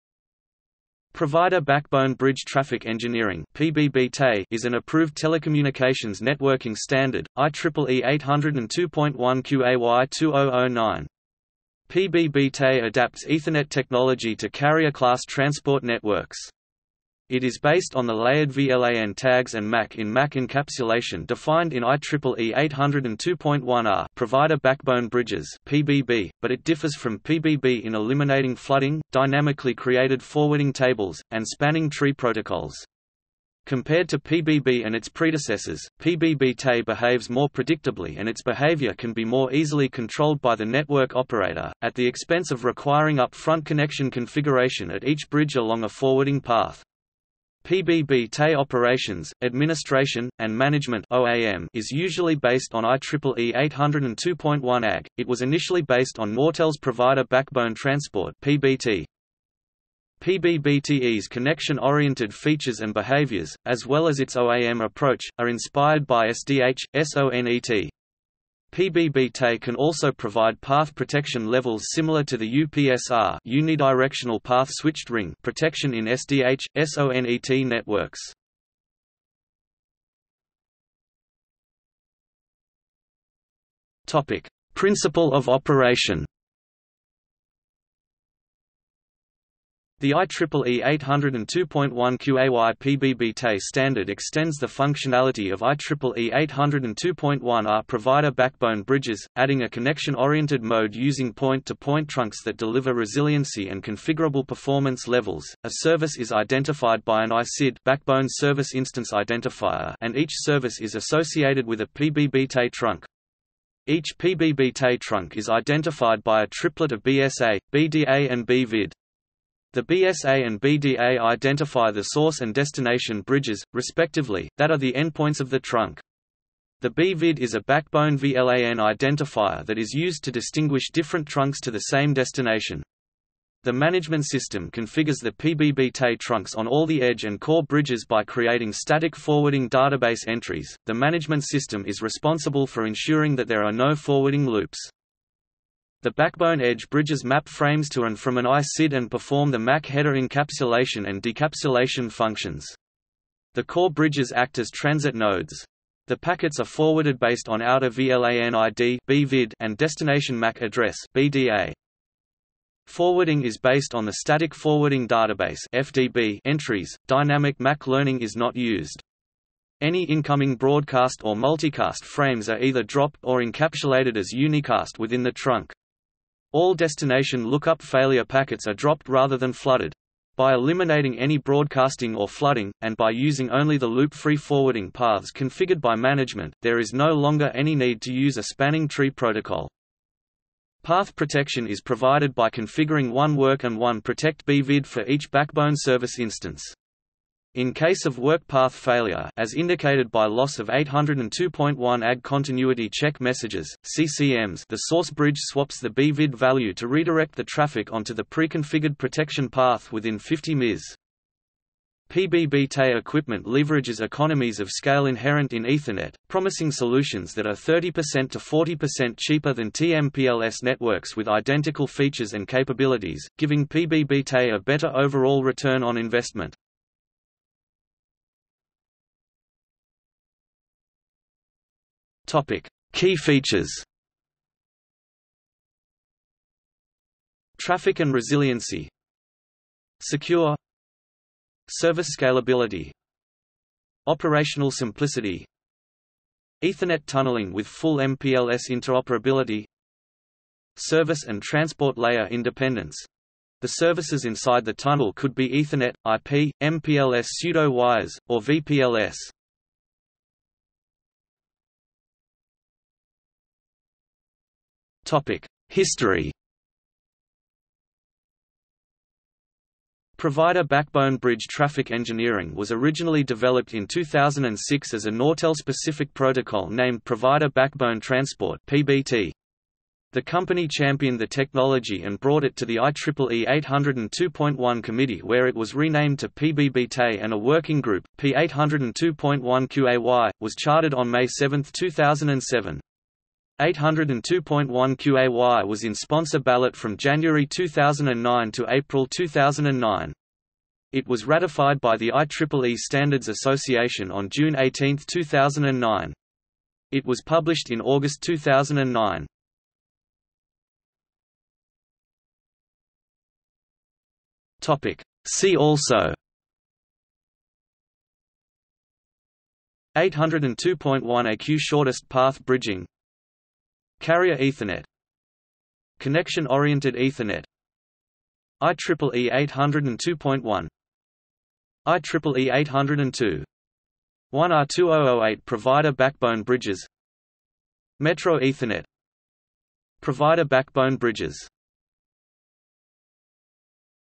Provider Backbone Bridge Traffic Engineering | Wikipedia audio article
00:01:20 1 Principle of operation
00:03:48 2 Key features
00:04:21 3 History
00:05:10 4 See also

- Socrates

SUMMARY
Provider Backbone Bridge Traffic Engineering (PBB-TE) is an approved telecommunications networking standard, IEEE 802.1Qay-2009.  PBB-TE adapts Ethernet technology to carrier class transport networks. It is based on the layered VLAN tags and MAC-in-MAC encapsulation defined in IEEE 802.1ah (Provider Backbone Bridges (PBB)), but it differs from PBB in eliminating flooding, dynamically created forwarding tables, and spanning tree protocols. Compared to PBB and its predecessors, PBB-TE behaves more predictably and its behavior can be more easily controlled by the network operator, at the expense of requiring up-front connection configuration at each bridge along a forwarding path. PBB-TE Operations, Administration, and Management (OAM) is usually based on IEEE 802.1ag.  It was initially based on Nortel's Provider Backbone Transport (PBT).
PBB-TE's connection-oriented features and behaviors, as well as its OAM approach, are inspired by SDH/SONET. PBB-TE can also provide path protection levels similar to the UPSR (Unidirectional Path Switched Ring) protection in SDH/SONET networks.